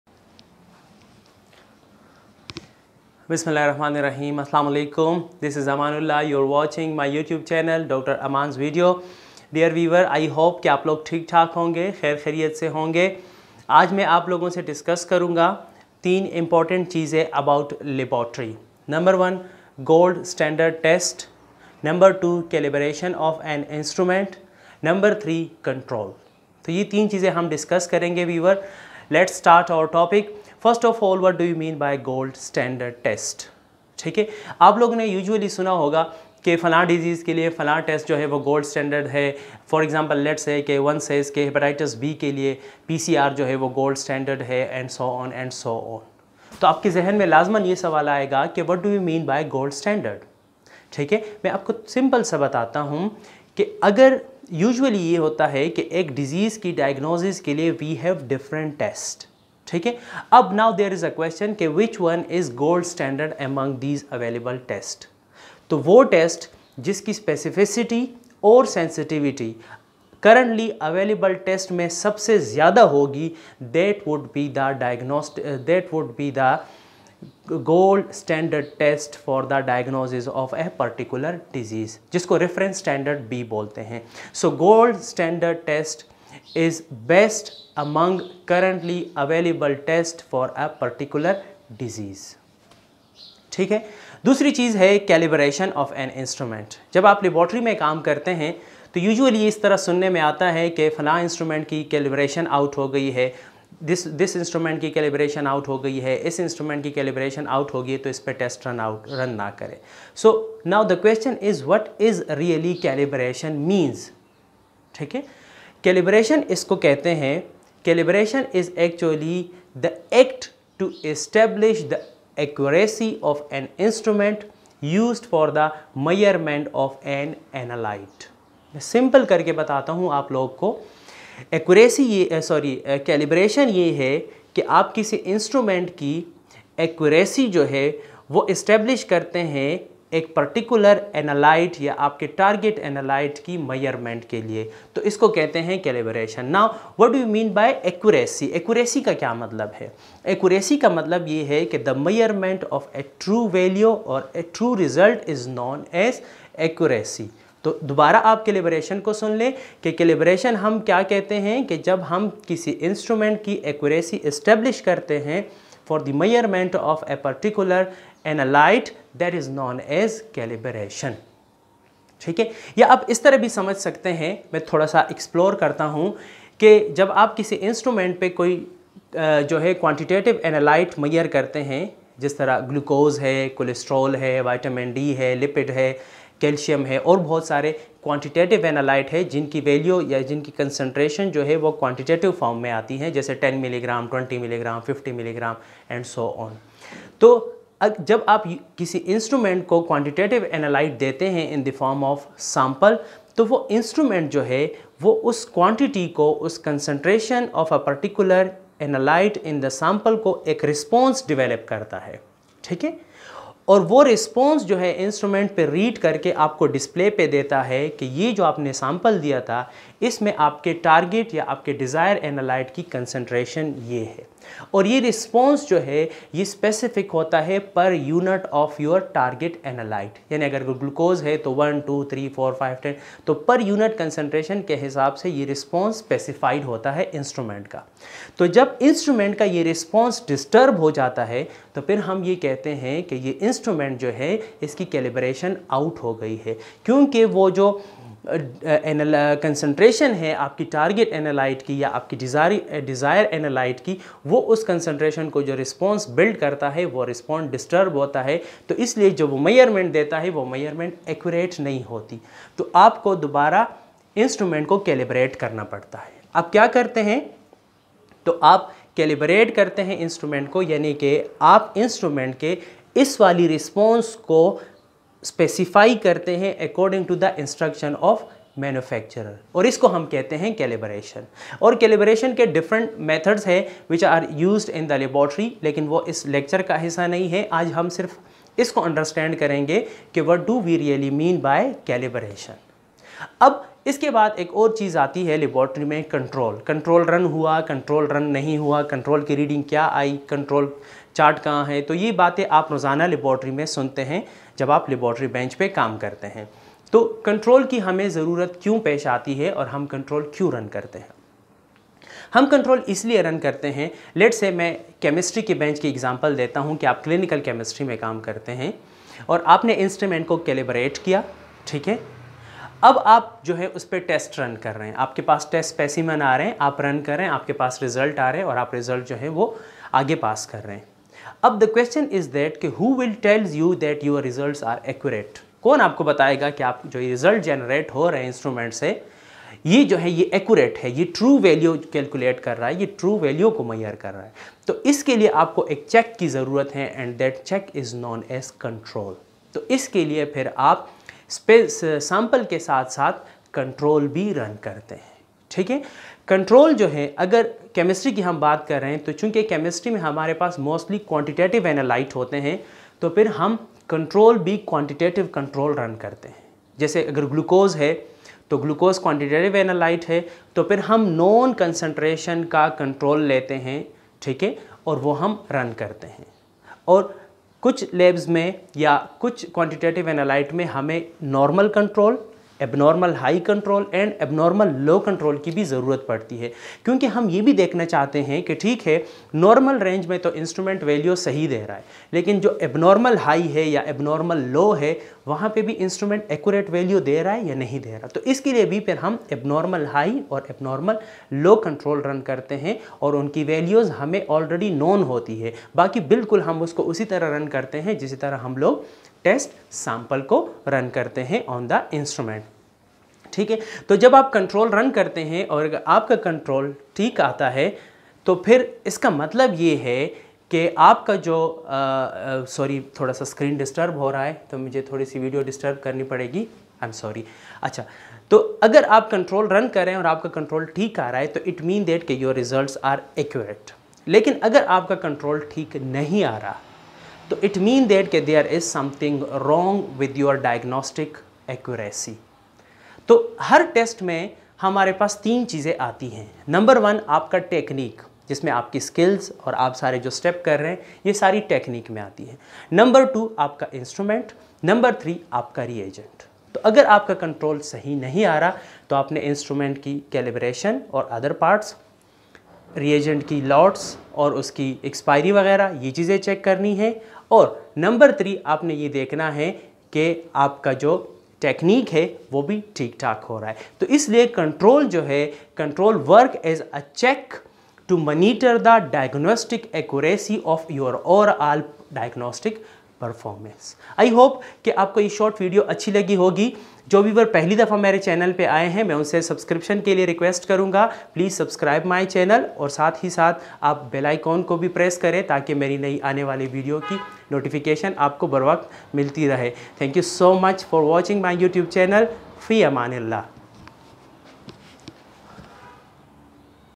Gold standard test | Reference test | Calibration of an instrument | Control in lab | Urdu-Hindi
This video lecture provide the base line information of Gold standard test, calibration, & control